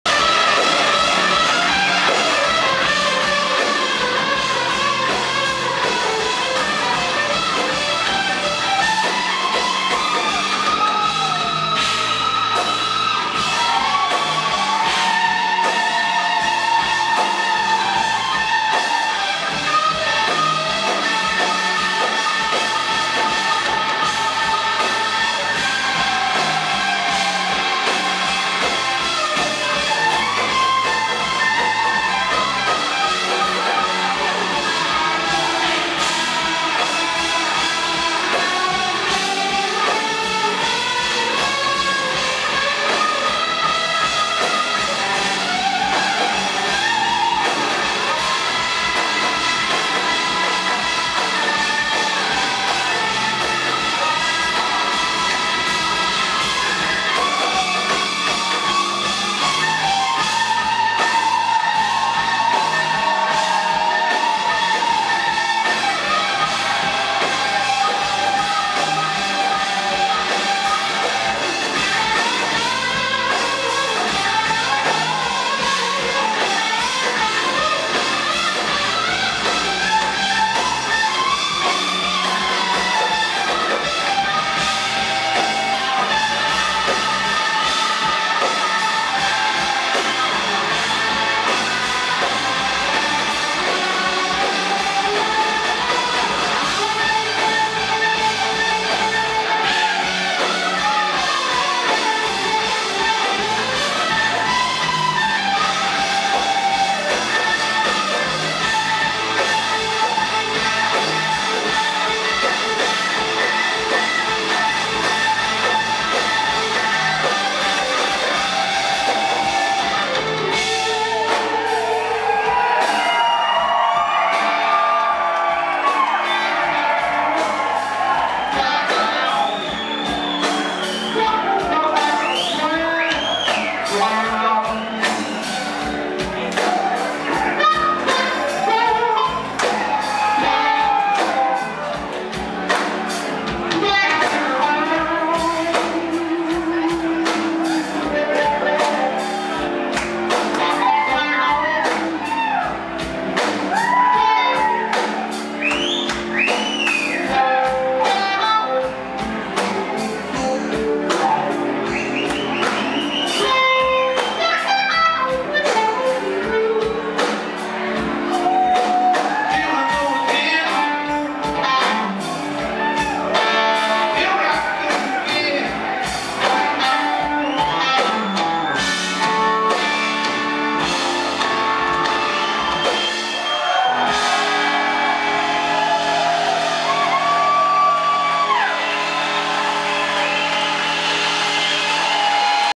Warren Haynes Band 11-09-11 Macon video #5
Killing it! These are numbered in the order that I shot them. I tried to be discreet & courteous, holding the camera low and/or blocking out the bright screen. This was a great show at the Cox Capitol Theater in Macon, Ga. Filmed with a Sony DSC TX-1 Cybershot digital camera.